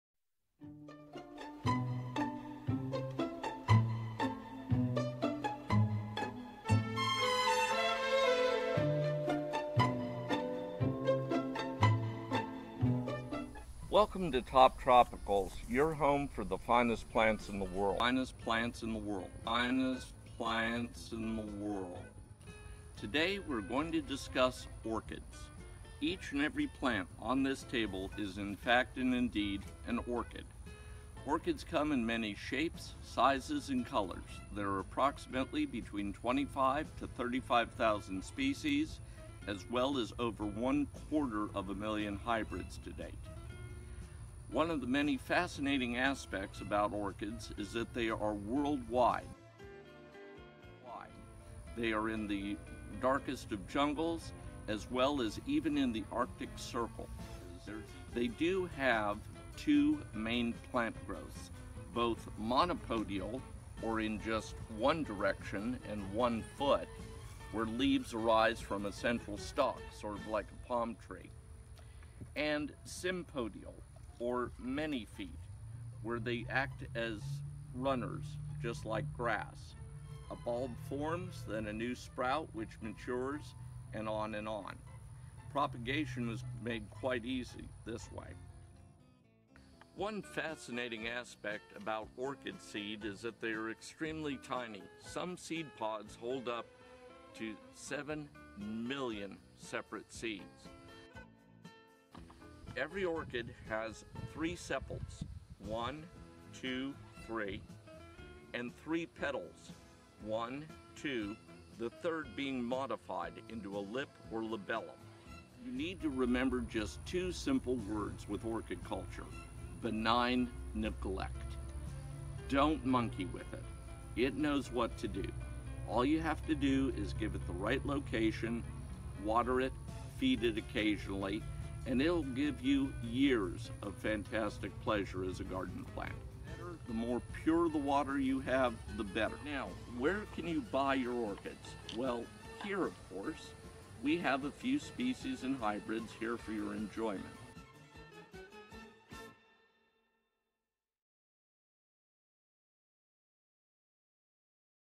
Orchids 101. Part 1. What is an orchid?
Orchids come in many shapes, sizes and colors. There are approximately between 25,000 species as well as over 250,000 of hybrids. It's one quarter of a million! Learn about these amazing plants from series of Orchid videos with our orchid expert Robert Riefer.